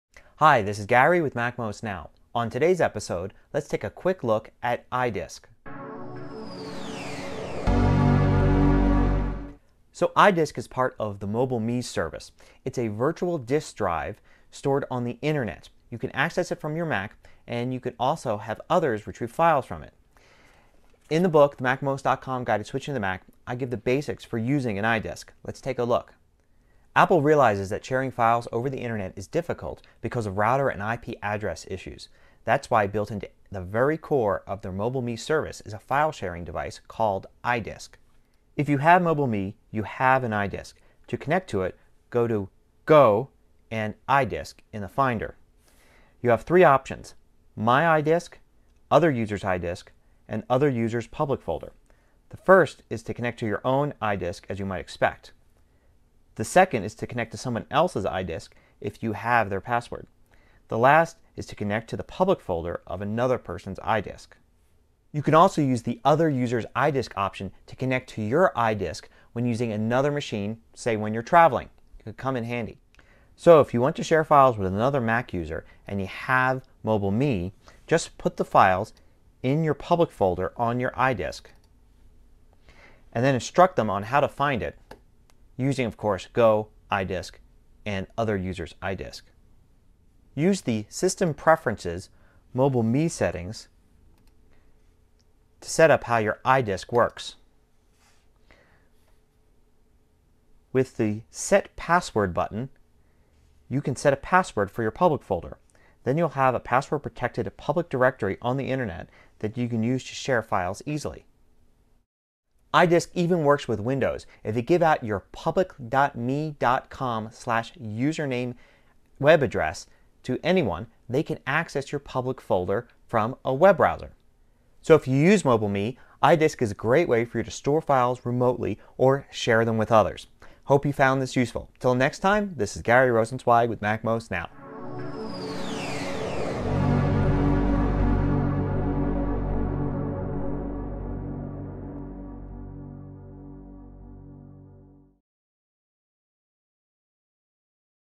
MobileMe iDisk Basics (MacMost Now 323)
If you use MobileMe, then you have an iDisk. It is a virtual hard drive on the Internet that you can use to store files and share them with others. Learn the basics of using your iDisk.